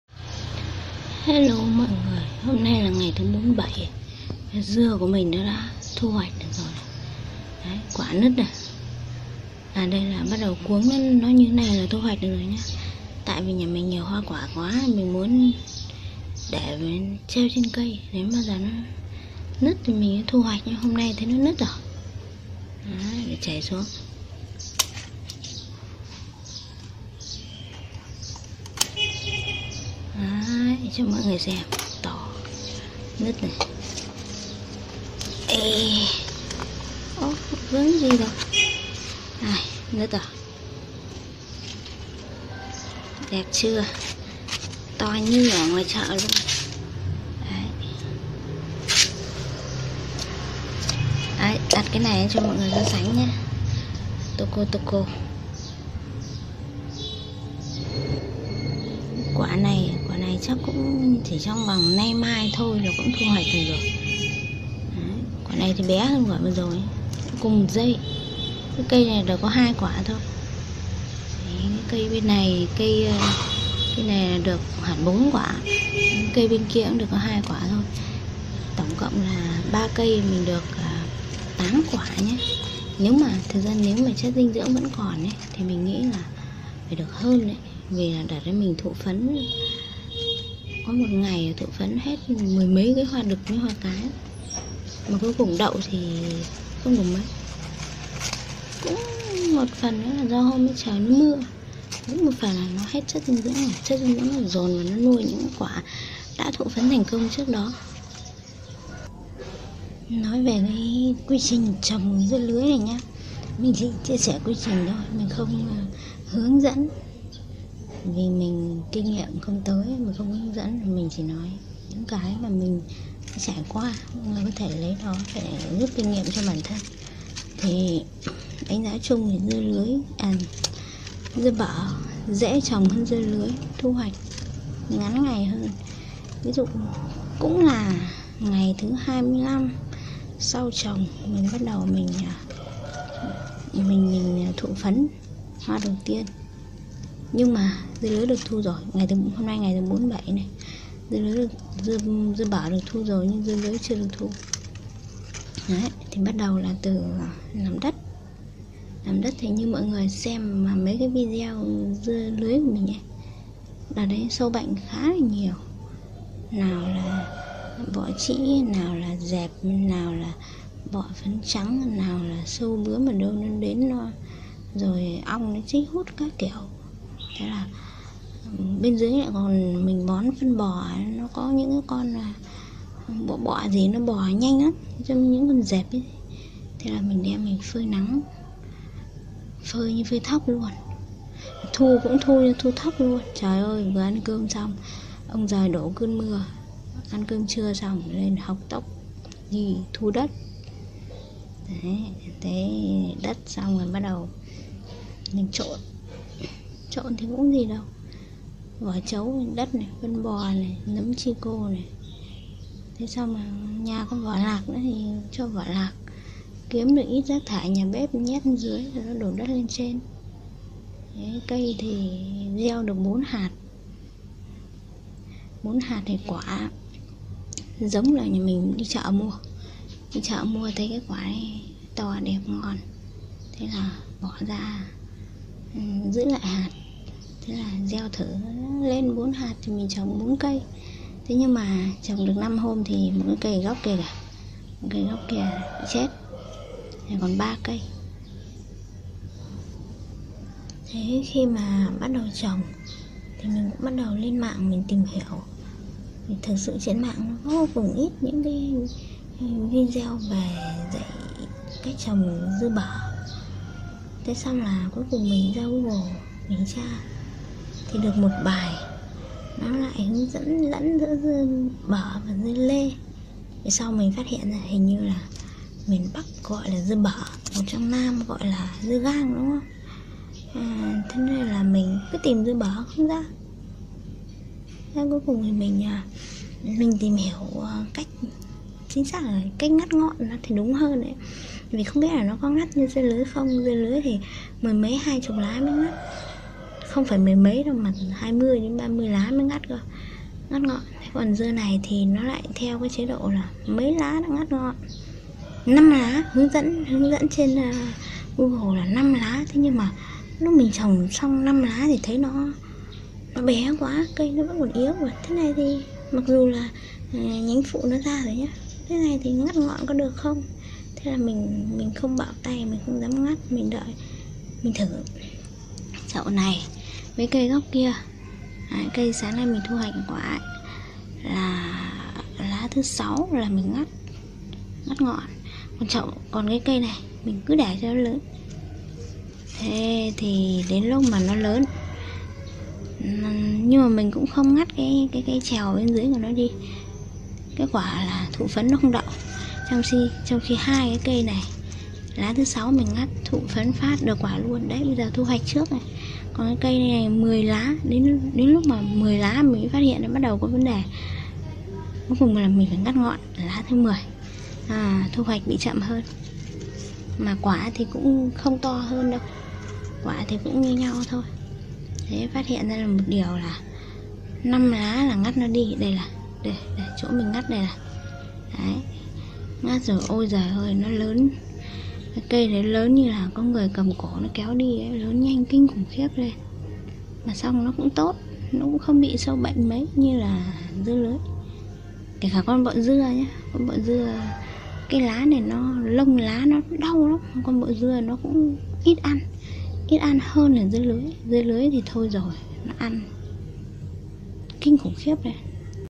Trồng dưa bở dễ lắm (P1) - Thời điểm bấm ngọn
Trồng dưa bở tưởng dễ, ai ngờ dễ thật!